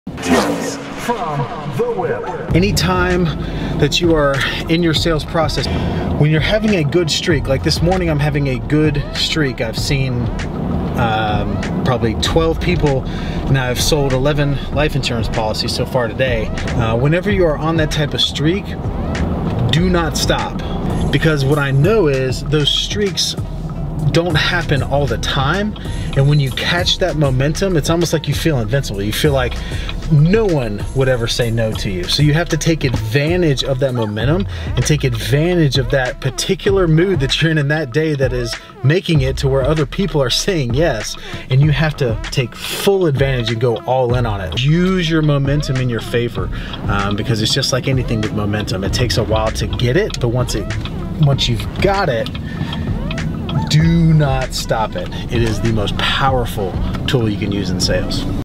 Use the Good Streaks | Tip From The Whip with Tyler Harris
When you're on a roll in your sales process, DON'T QUIT!